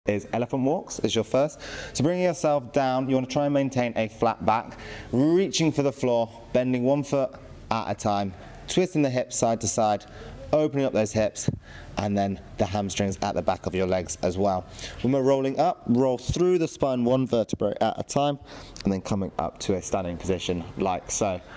Elephant Walks
The half split is a yoga pose that stretches the hamstrings and calves and can improve balance and stability. Watch Personal Trainer and Yoga Teacher Ryan demonstrate how to perform half split here.

How To Do A Half Split
1. Begin in a kneeling position and step one foot forward into a lunge.
2. Lean over your front thigh to place both hands on the floor.
3. Step the foot forward again and begin to sink your hips down and back while straightening the front leg.
4. Flex the front foot.
5. You can either hinge over the front leg with a flat back, fold over the leg allowing your back to round, or bring your torso upright and balance in this position.
6. Hold for 30 seconds or as long as comfortable.